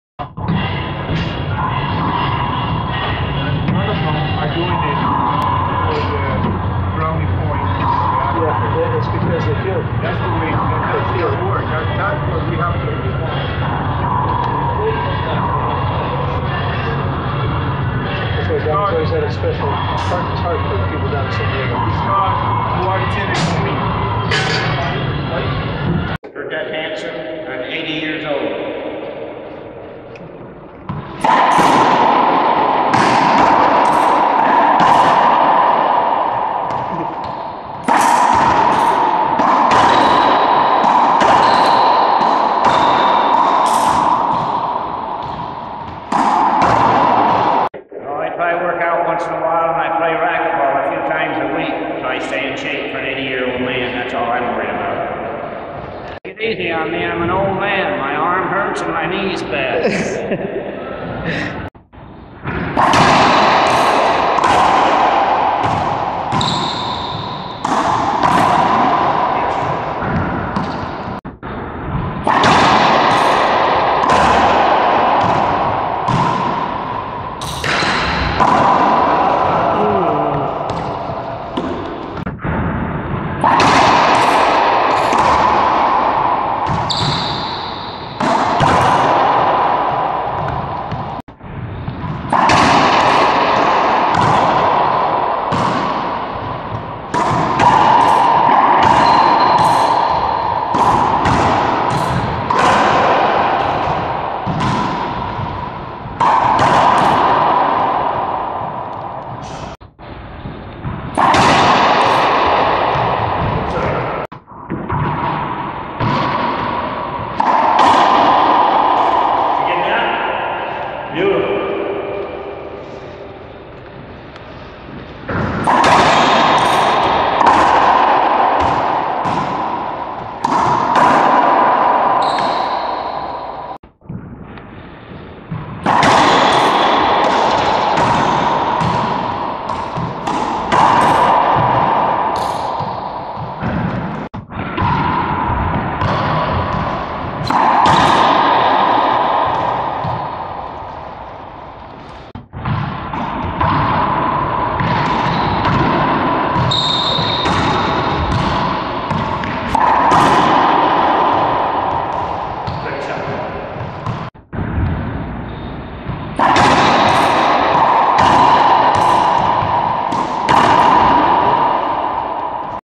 80 Year Old Racquetball Player
80 year old Burdette Hansen plays racquetball at L.A. Fitness Chula Vista,CA. He amazes everyone  for staying in such a good shape, quickness and sharp shots beating young players. He deceives the enemy by playing the "old man" role.